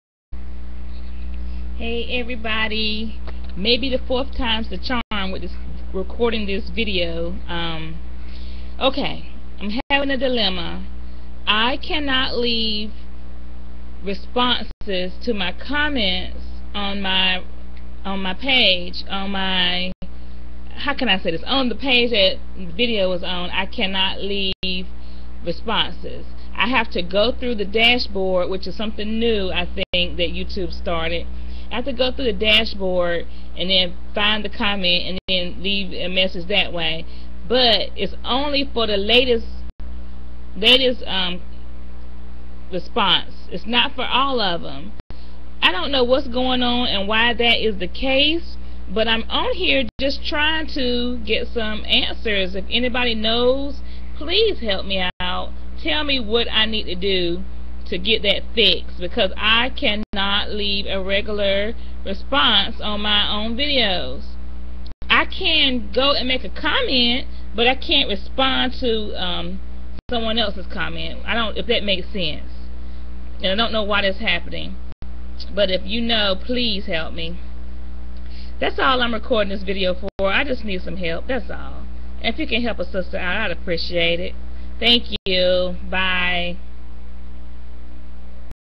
Help!! I can not respond to comments on my videos!
If you can tell me why I am no longer able to respond to comments on my videos. please help! Thank you